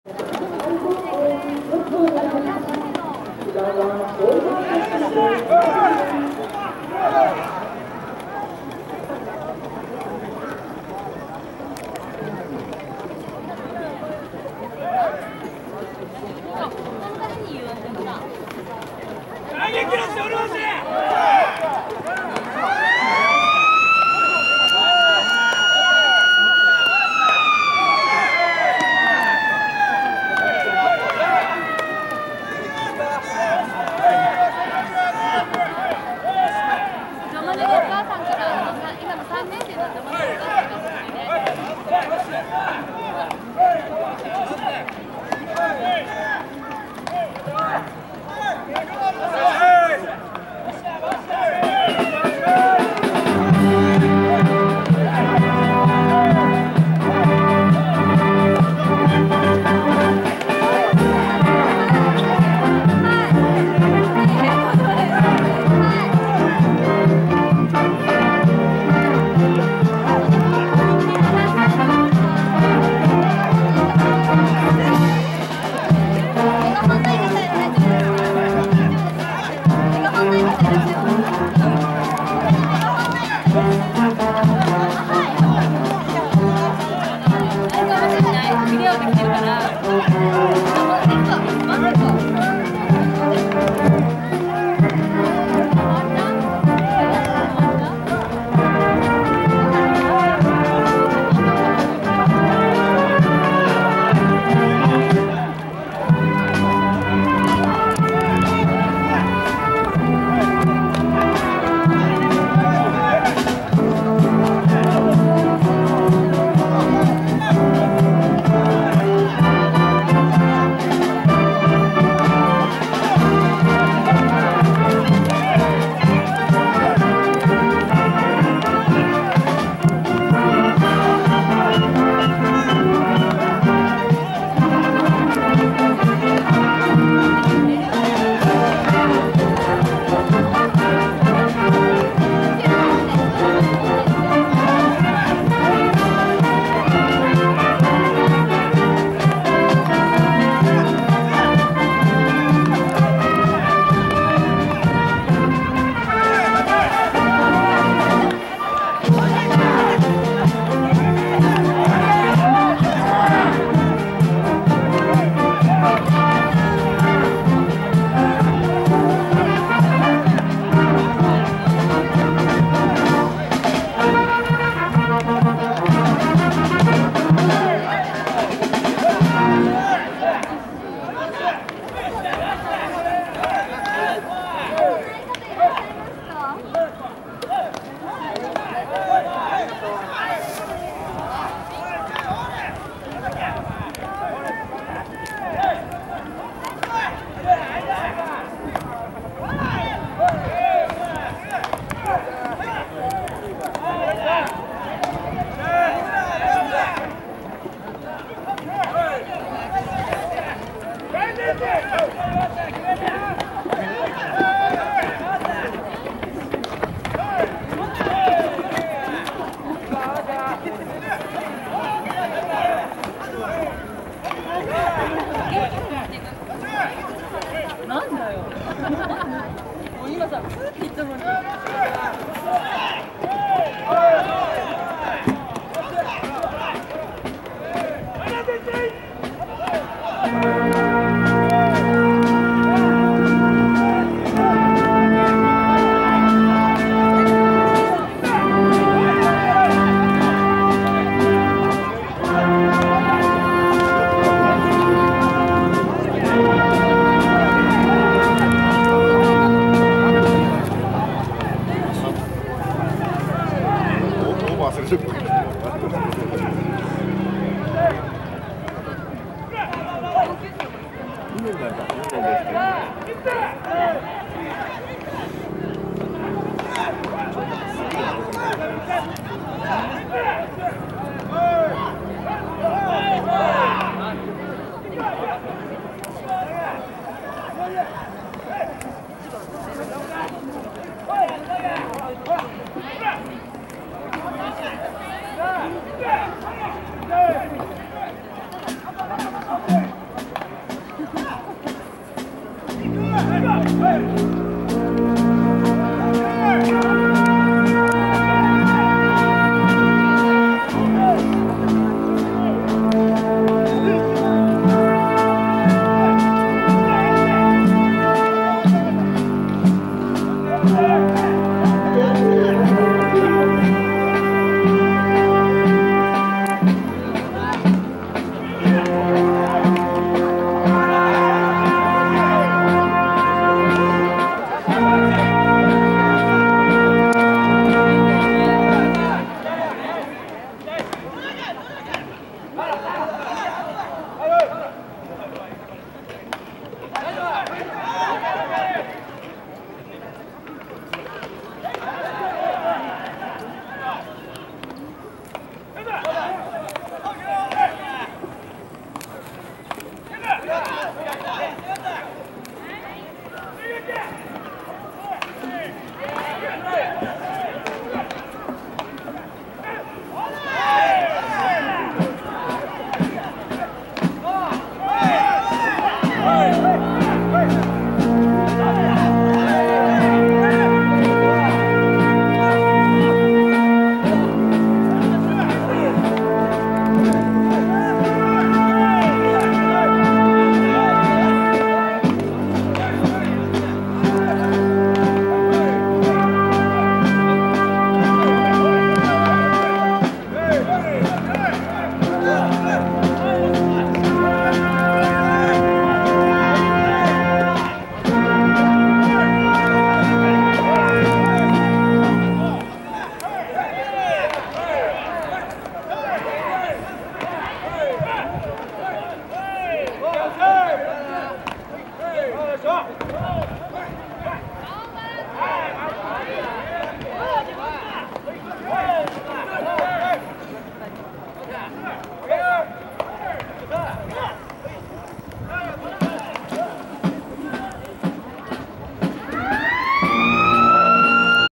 花咲徳栄 シートノック 141002
141002　市営大宮球場
埼玉県大会　準々決勝　昌平戦にて